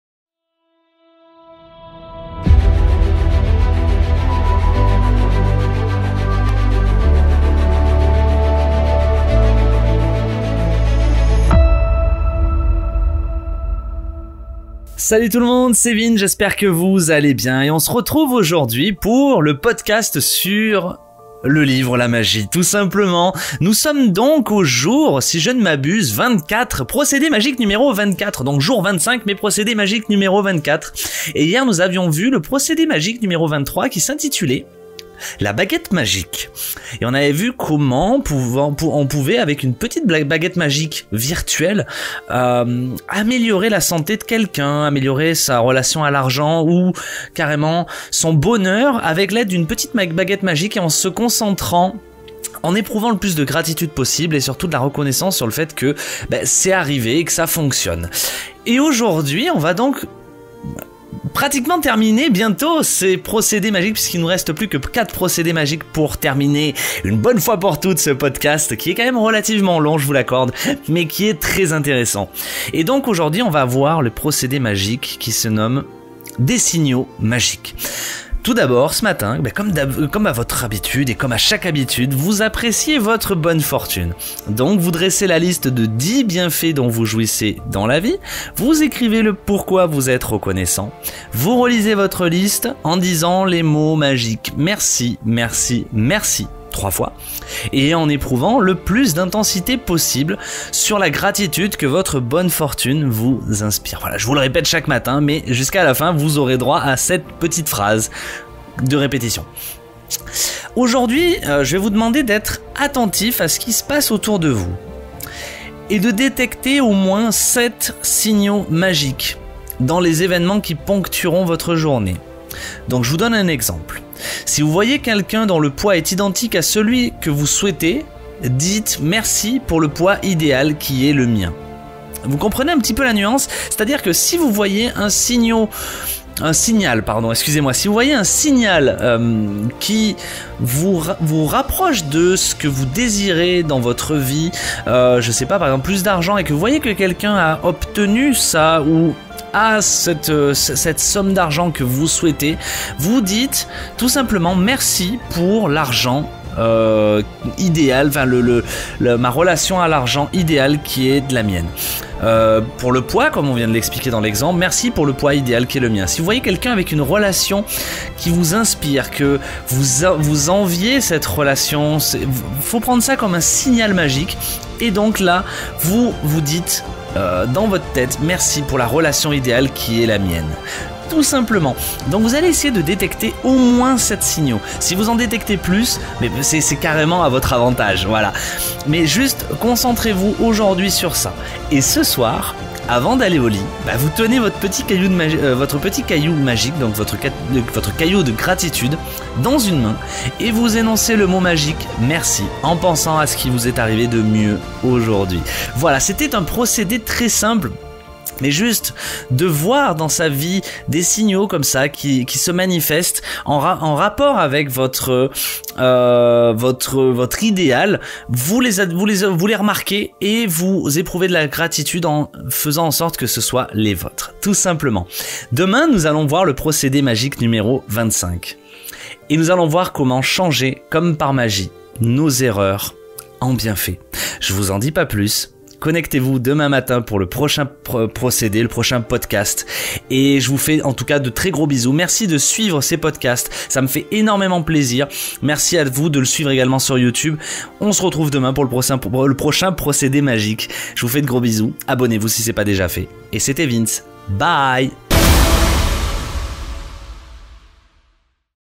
Jour 24 - Des signaux magiques !
Et si vous pouviez devenir ou redevenir d'avantage Positif?! C'est ce que je vais tenter de faire au travers de ce podcast ...

Il est donc temps de commencer cette première série par un livre qui m'inspire encore aujourd'hui. Le livre de Rhonda Byrne intitulé "La Magie"

Au travers de ce livre, nous découvrons 28 techniques de gratitude à mettre en place pour faire entrer la magie dans sa vie!

On se retrouve donc aujourd'hui pour parler de la vingt quatrième technique du livre la magie qui se nomme: Des signaux magiques !

On se retrouve dans le prochain épisode pour découvrir la technique de comment changer comme par magie vos erreurs en bienfait.

Des bisous